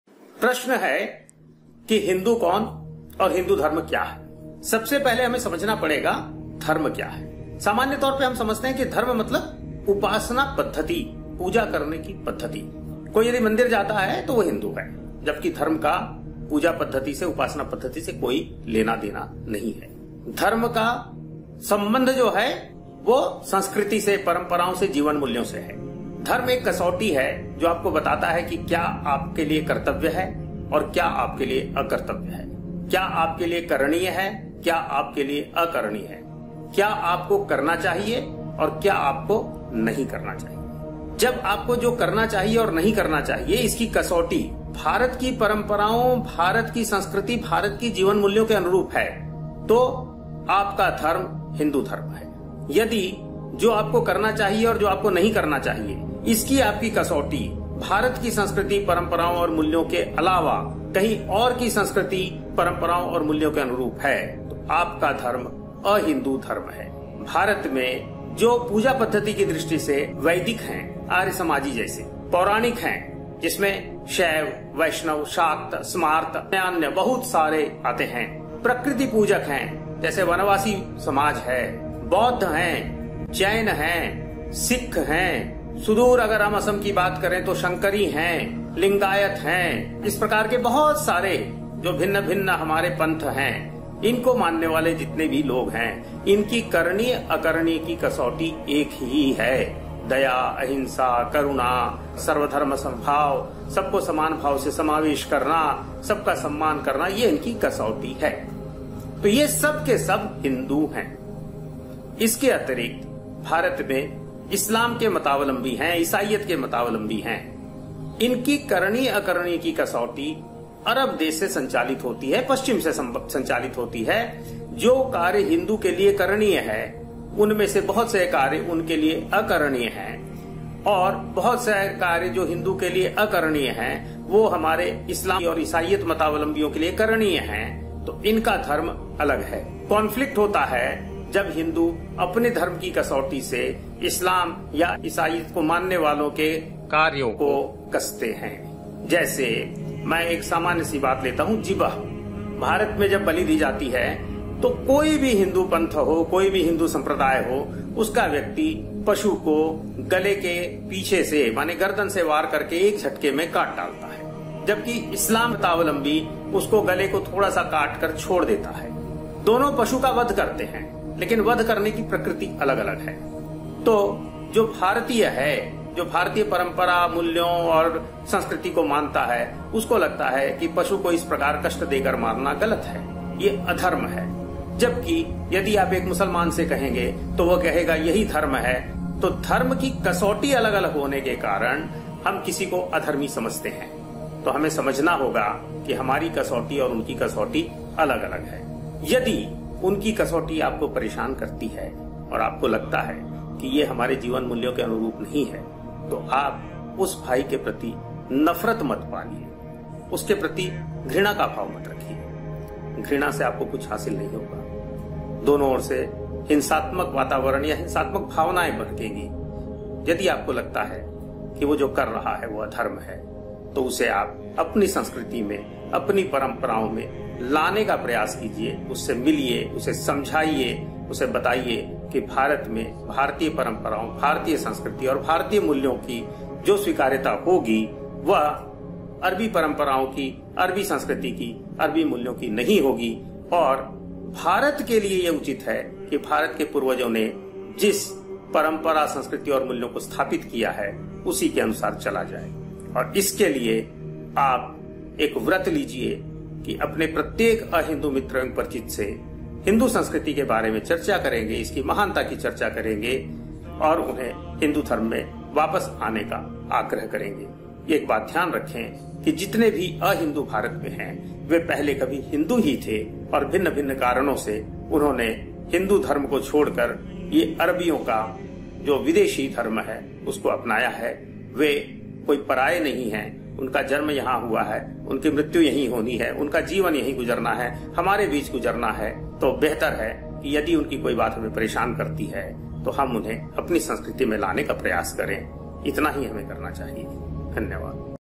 Sunil Agrawal on Dharma, Hindu and Non Hindus
धर्म का मतलब क्या? हिन्दू धर्म का मतलब क्या? कौन अहिन्दू? कनफ्लिक्ट का कारण क्या? उपाय क्या? जानने के लिए देखिए